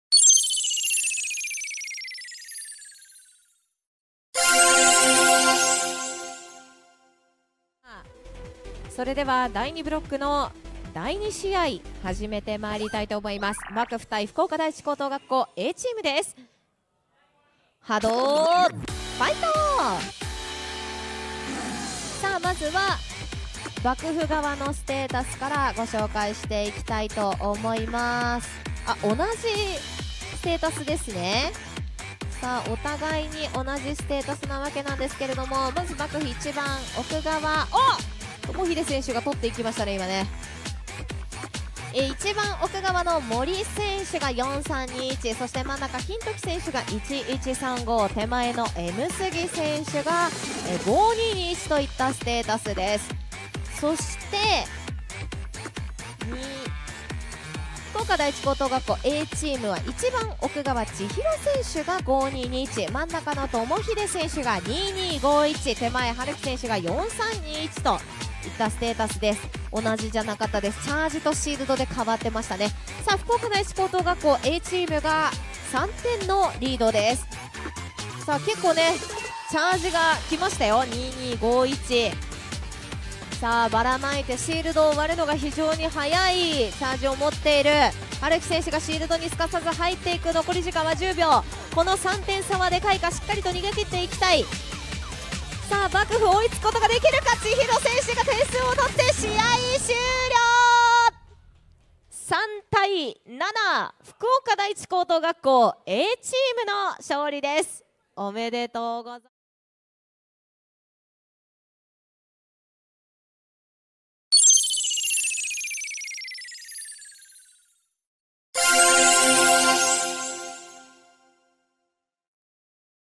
【HADO 2024】 ADVANCE CUP #56 #4 ② BAKUFU VS 福岡第一高等学校Aチーム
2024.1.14 お台場で行われた　＠HADO  ADVANCE CUP #56